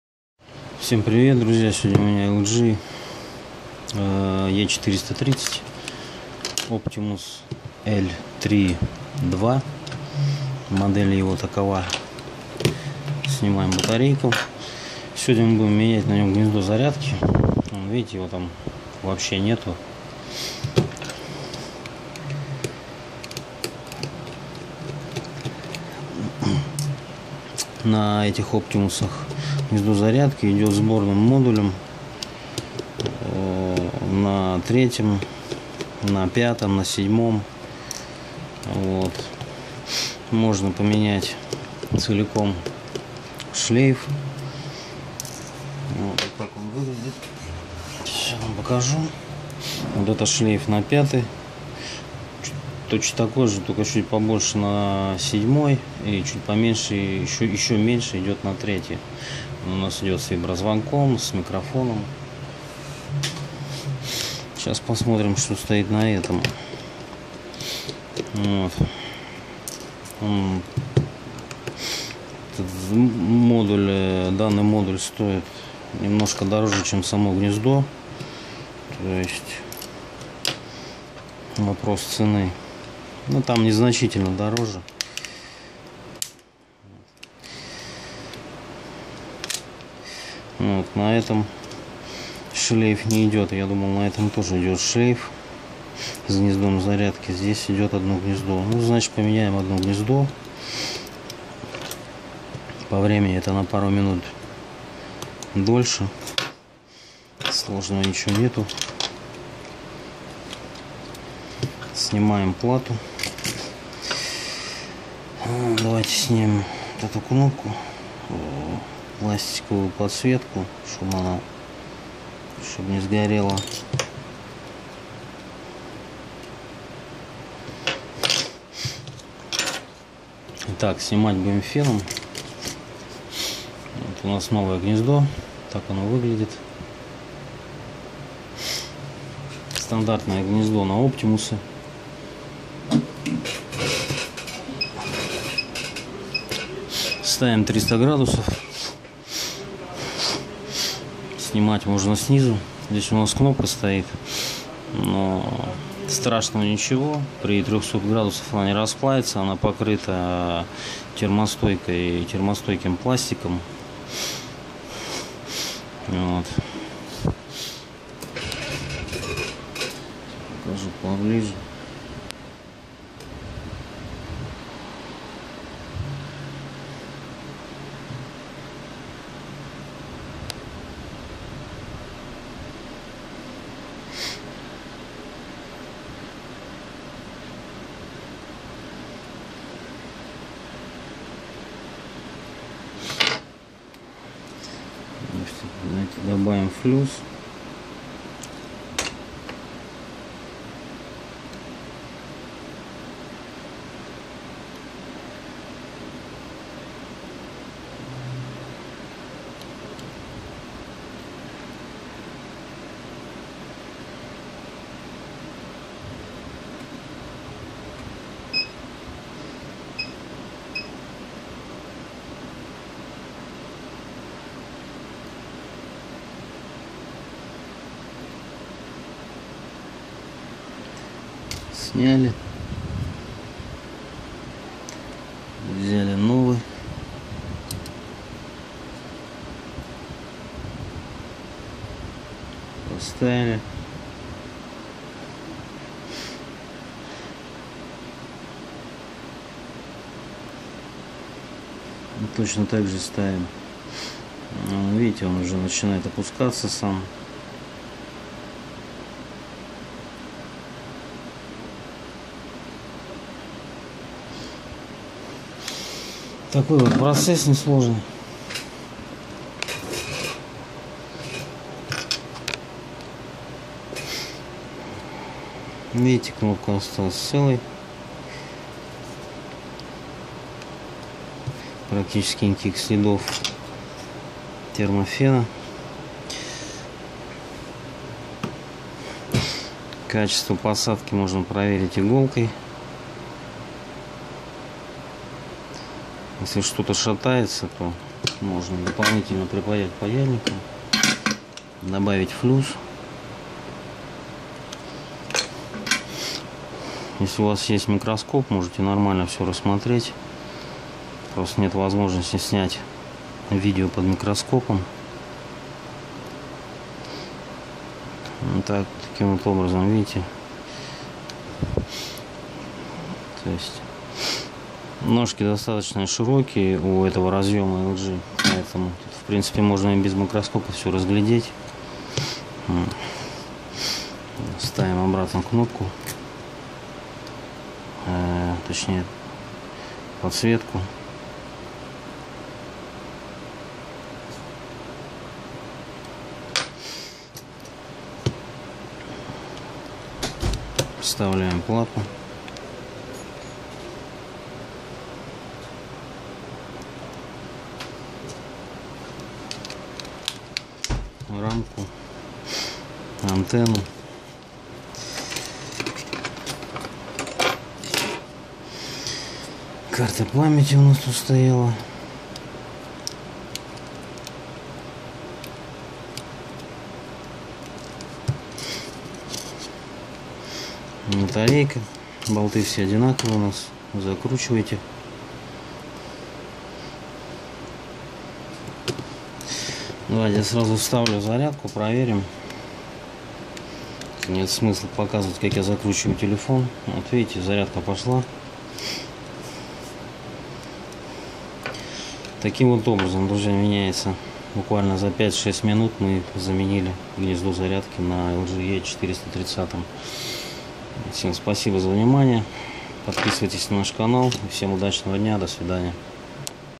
LG E430 Optimus L3 II не заряжается. замена гнезда зарядки
Smartphone (8,1 cm (3,2 Zoll) Touchscreen, 1GHz, 512MB RAM, 3,2 Megapixel Kamera, Android 4.1)